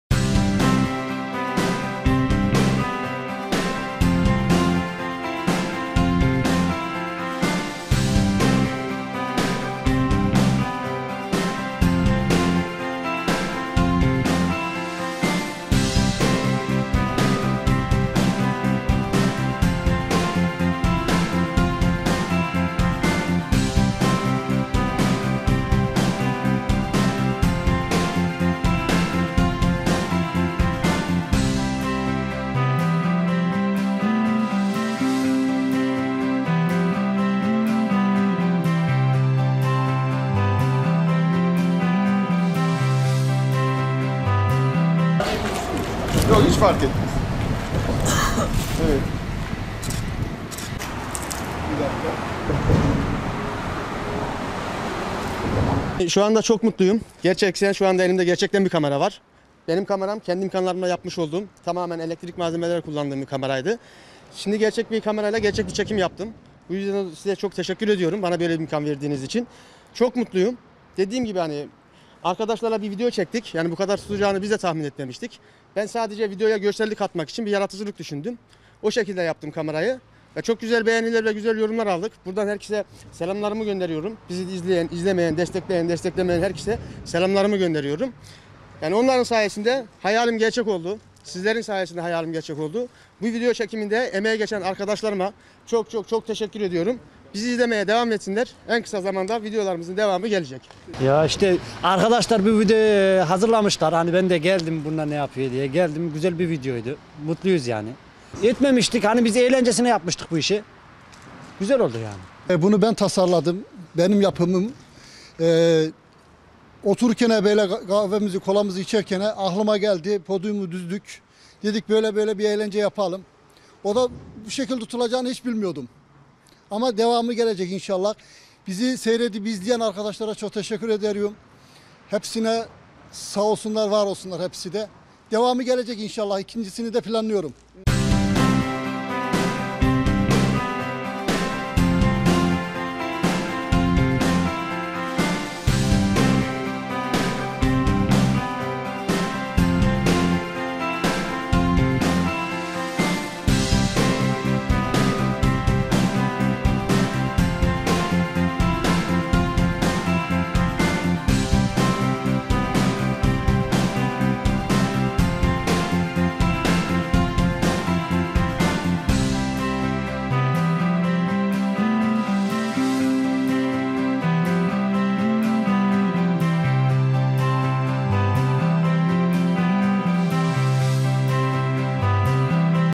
İnşaatta defile yapan işçiler sosyal medyada dikkat çekti
◥Trabzon’un Araklı ilçesinde yapımı devam eden Hükümet Konağı inşaatında çalışan işçiler sosyal medyada izlenme rekoru kıran bir video çekti.

◥Strafordan yaptıkları podyuma çıkan işçiler çalışma kıyafetleri ile birlikte ellerine aldıkları mala, keser, kazma ve çimento mikseri ile yürüdü.

◥Ünlü mankenlere taş çıkartacak şekilde yürüyen işçilerden en çok dikkati ise elindeki demir ve arkasından sarkan kablo ile arkadaşlarını görüntülemeye çalışan kameraman çekti.

◥Şarkı eşliğinde sosyal medyaya yüklenen video 2 günde 5 milyon kişi tarafından izlendi.

◥Eline aldığı demir parçasıyla kameramanlık yapan inşaat işçisi Tuncay Şahan, yaptığı açıklamada “Arkadaşlarla bir video çektik bizde bu kadar tutacağını tahmin etmemiştik. Ben videoya nasıl bir görsellik yaparım diye düşündüm. Aklıma kameramanlık geldi. Çok güzel yorumlar aldık. Bizi izlemeye devam etsinler. En kısa zamanda yeni videolarımız gelecek” dedi.